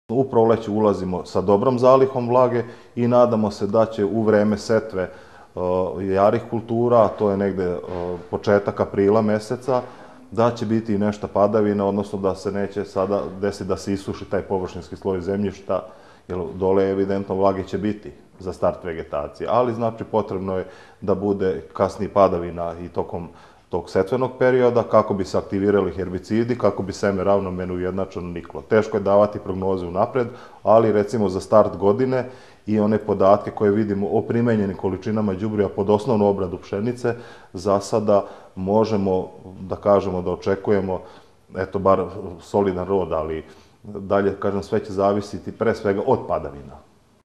Potrebna prihrana
Najvažniji posao koji očekuje poljoprivrednike jeste prihrana ozimih strnina.

Ekipe subotičke Poljoprivredne stručne službe analizirale su prethodnih dana učešće azota u zemljištu. Na 64 uzorka sa 470 hektara zaključeno je da se na dubini od 0 do 90 centimetara nalazi oko 55 kilograma azota, te je potrebno prihranjivanje pšenice sa 110 do 120 kilograma čistog azota po hektaru. Po pitanju vlage, analize pokazuju da su se spojile gornja i donja vlaga, a nakon topljenja aktualnih snežnih padavina treba iskoristiti mrazna jutra i primeniti UREA-u.

Boris Šuman